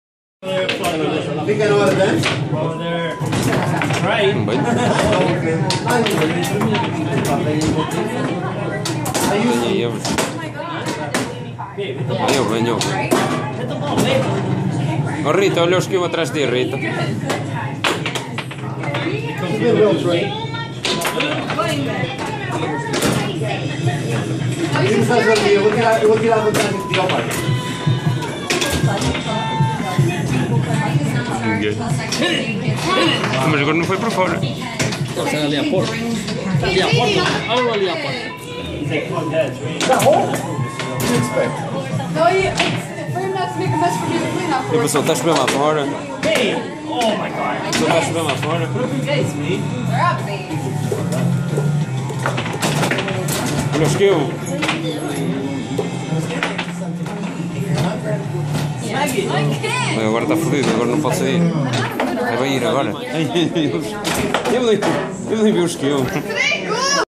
Squirrel eating peanuts in bar..
This squirrel has no shame,  comes in eats all the peanuts and doesn't even buy a drink...